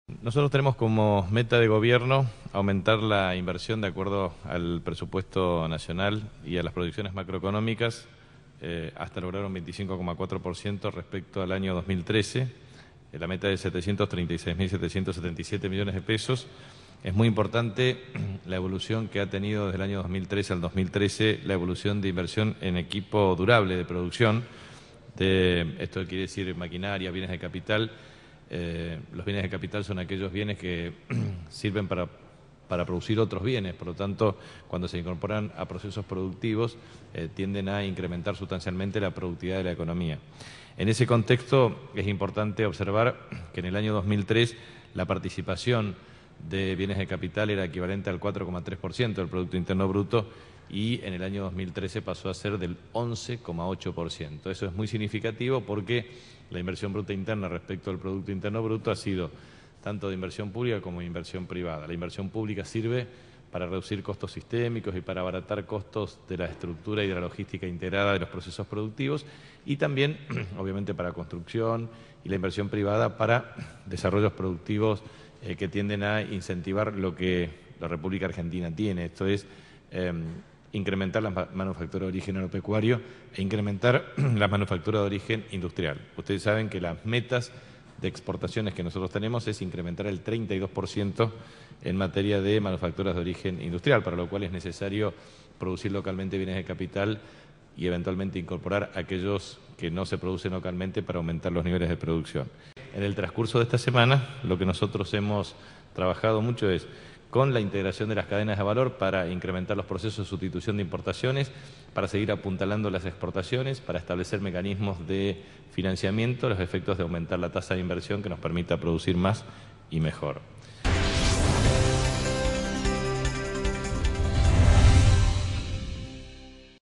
El Gobierno buscará incentivar la producción nacional de autopartes
El jefe de Gabinete, Jorge Capitanich, aseguró esta mañana que el gobierno nacional evalúa generar "el incentivo necesario para la producción de autopartes" en el país, de manera tal de "disminuir sustanciamente el volumen de demanda de divisas".
En ese marco, en su habitual contacto con la prensa en la Casa de Gobierno, Capitanich indicó que la presidenta Cristina Fernández de Kirchner instruyó para que avancen en ese sentido a la ministra de Industria, Débora Georgi; el ministro de Economía, Axel Kicillof, y a él mismo.

Capitanich destacó las reuniones que viene manteniendo esta semana con los distintos integrantes de la cadena de valor e inversores que quieren establecerse en el país.

"Esta semana estuvimos trabajado mucho con la integración de la cadena de valor para la sustitución de importaciones, aumentar las exportaciones, establecer mecanismos de financiamiento para aumentar la tasa de inversión y producir más y mejor", dijo y remarcó el impacto que las nuevas inversiones tienen en la "generación de empleo".